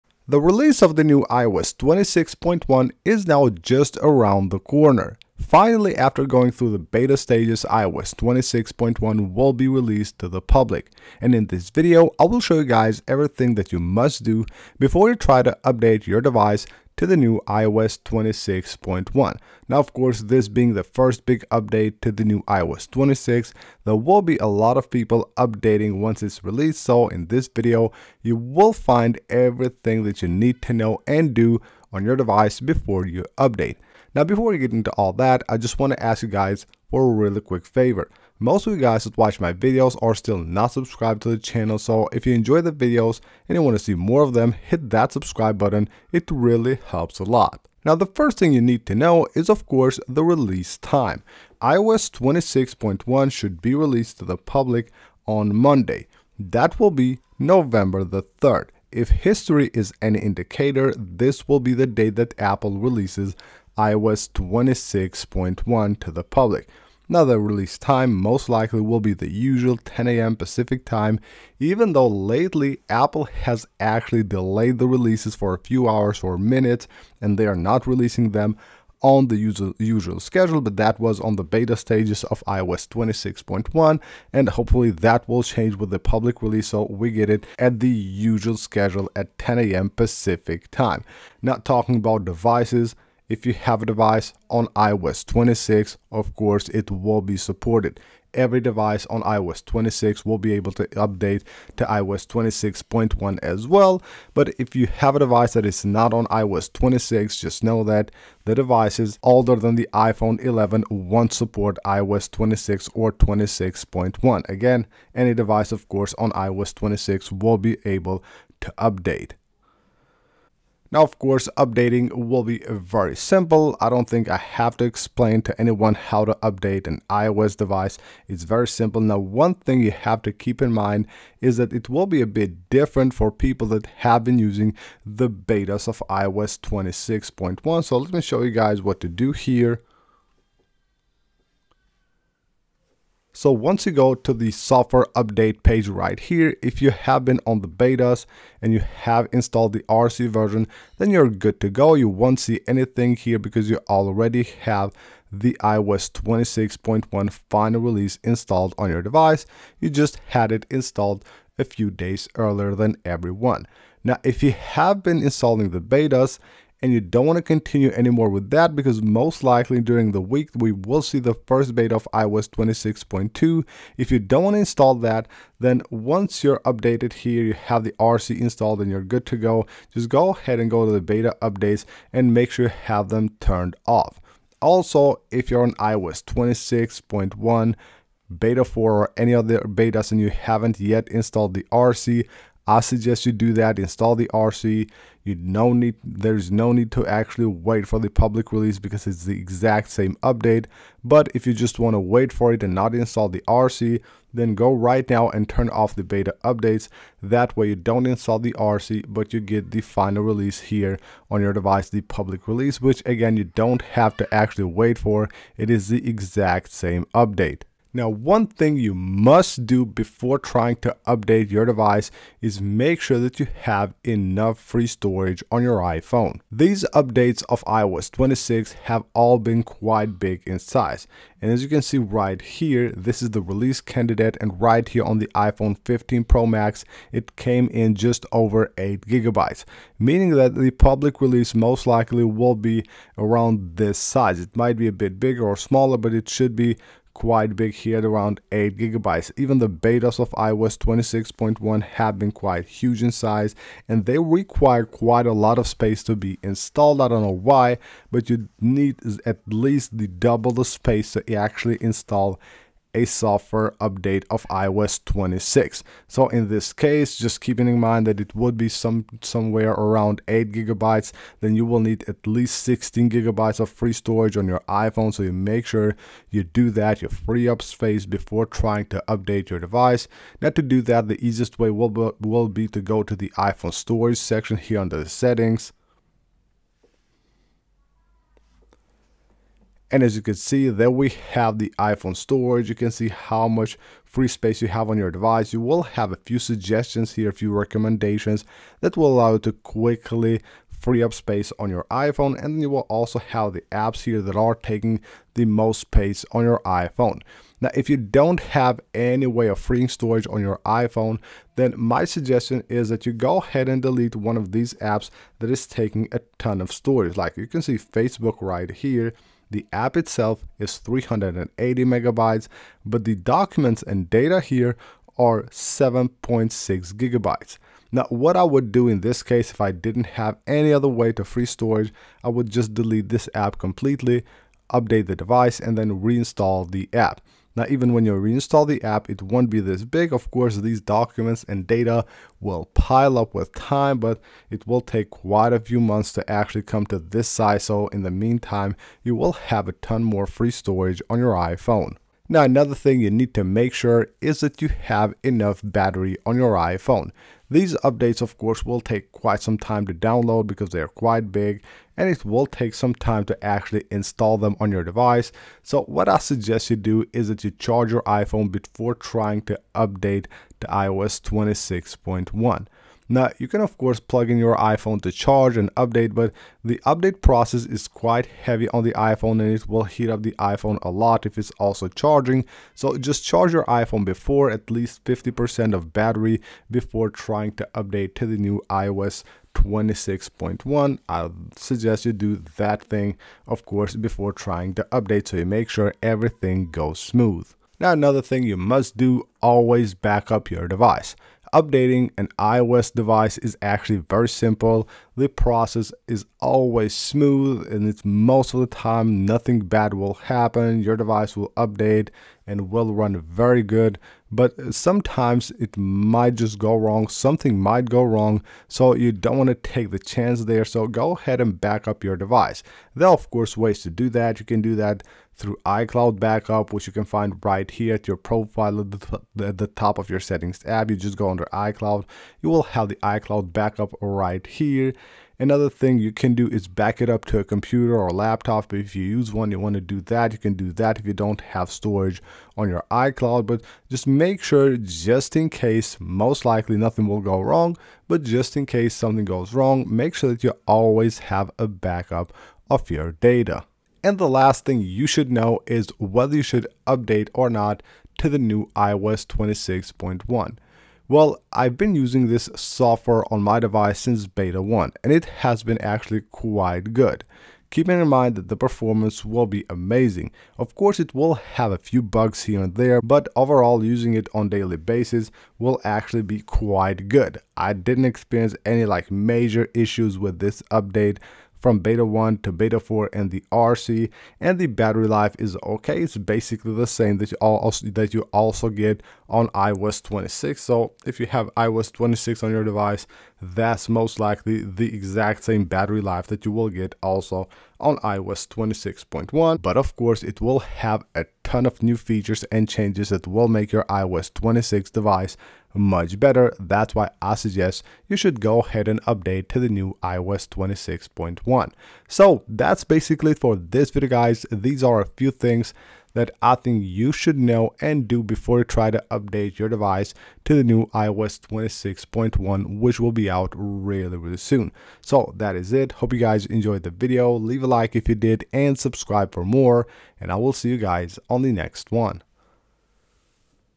iOS 26.1 - Do this Before You Update !
iOS 26.1 will be released soon to the public with amazing new features and changes.

In this video you can learn a few things you must do before you update your iPhone to iOS 26.1

Release 0:00
Devices 1:31
Beta 2:00
Storage 3:37
Charge iPhone 5:52
Backup 6:37
Should you update 7:33